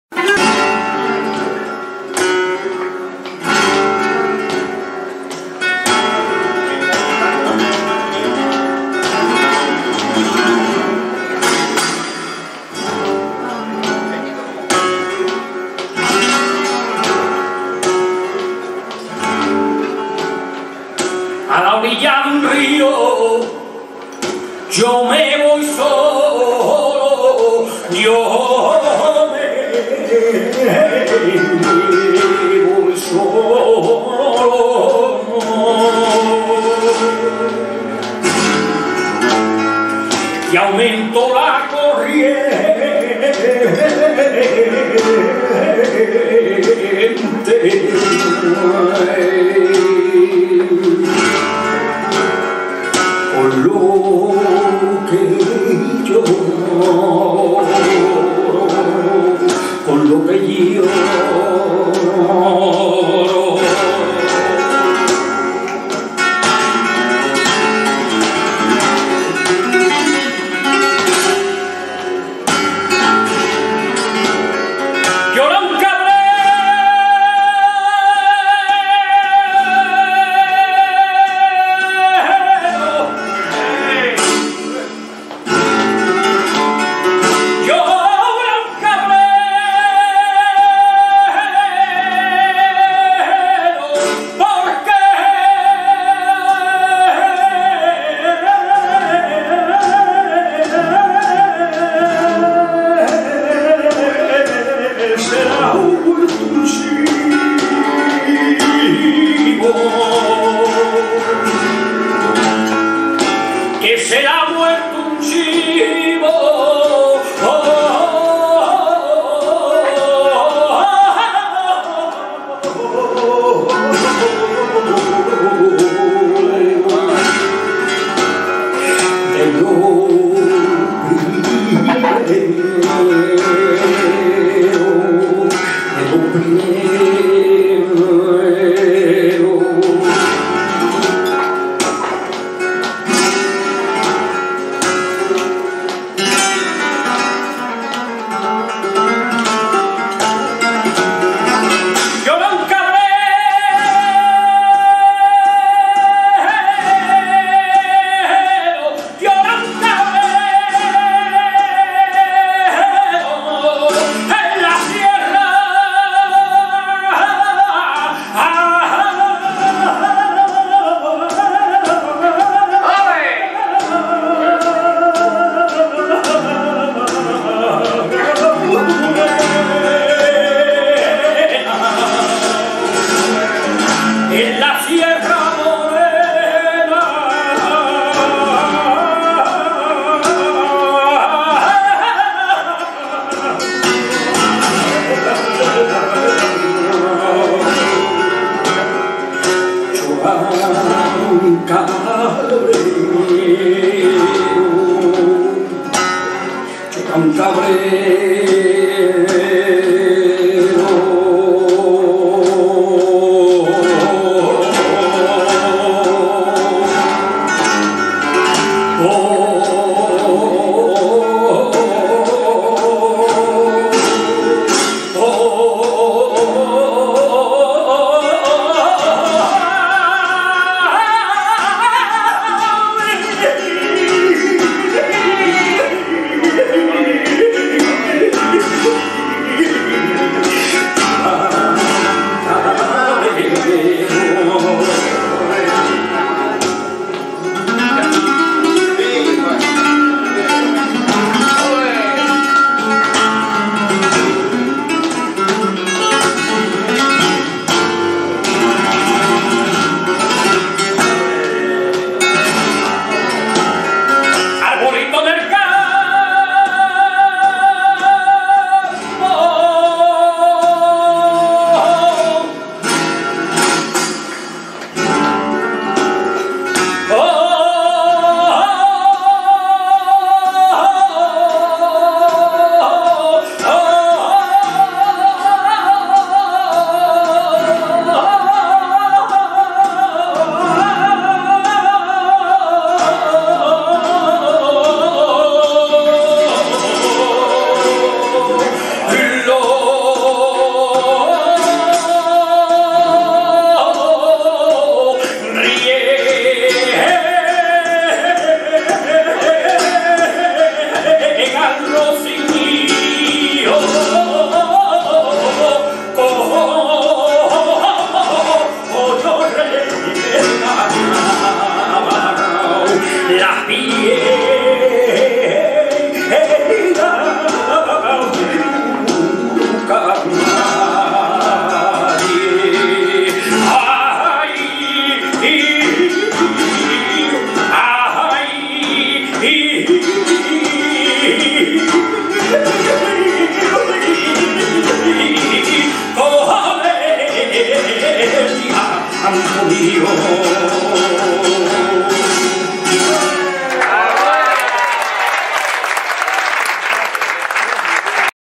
Conil de la Ftra. (Cádiz) JUAN PINILLA en la Guitarra Flamenca SERRANA
Al cante: Juan Pinilla
Al toque: José Fermín Fernández
Juan Pinilla fue Lámpara Minera de los Cantes de la Unión en 2007.
José Fermín Fernández fue Bordón Minero de la Unión en 2019.